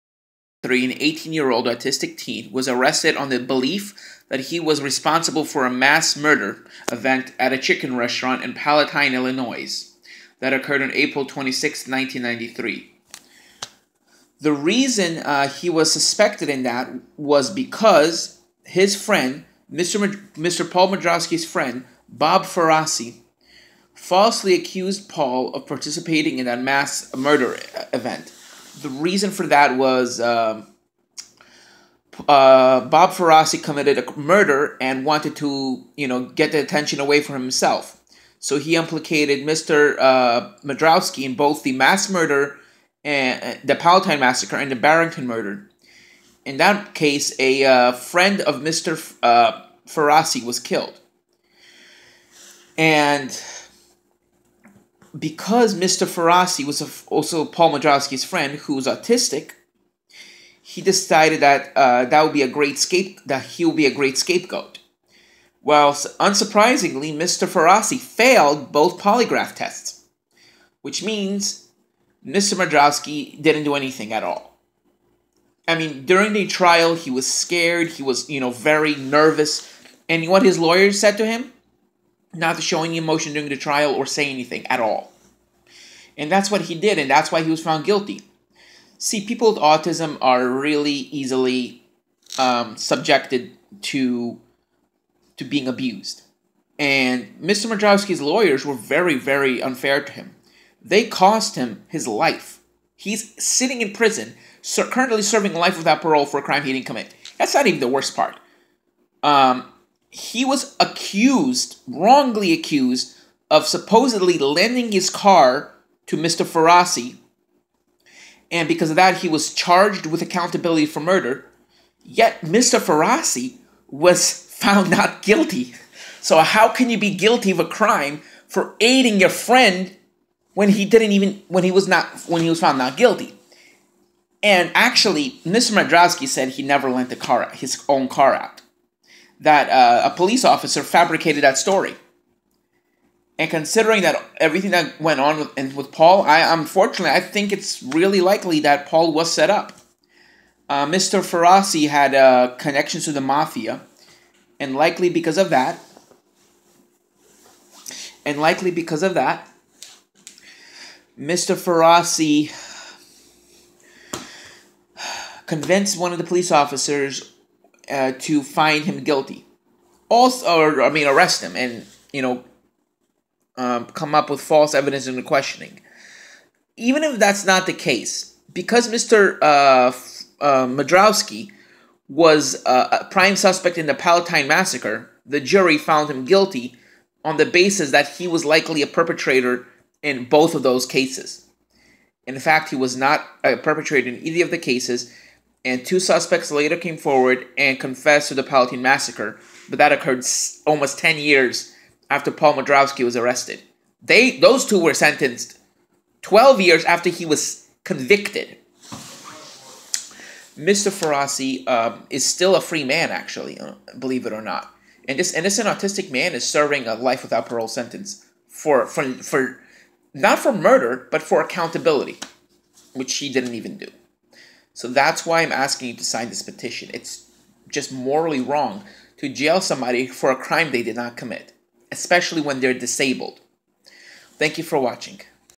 Autism discrimination
Please sign this petition for Paul Modrowski, an innocent autistic man given a life without parole sentence.